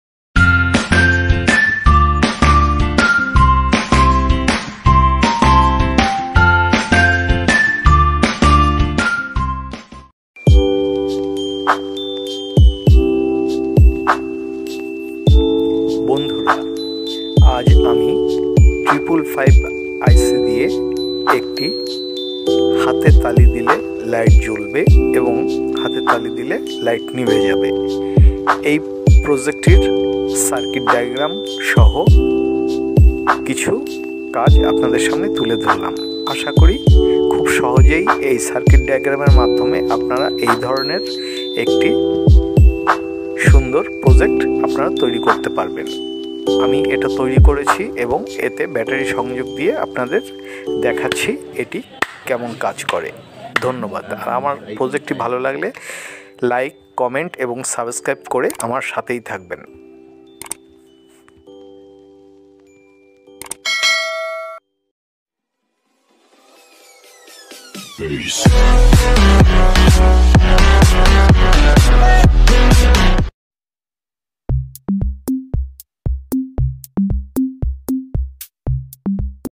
হাততালি দিলে জ্বলবে লাইট।clap switch circuit project diagram.
বন্ধুরা এখানে একটি হাতে তালি দেওয়া সার্কিট ডায়াগ্রাম তুলে ধরা হয়েছে যেটির মাধ্যমে খুব সহজে যেকোনো ডিভাইস অন অফ করতে পারবেন শব্দ নিয়ন্ত্রিয়ন এর মাধ্যমে।